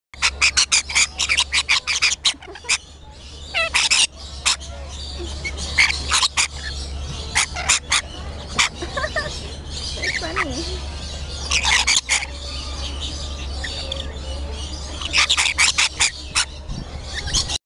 Lorikeats at Aquarium of the Pacific - feet biting
Lorikeats at Aquarium of the Pacific - one bites the other's foot!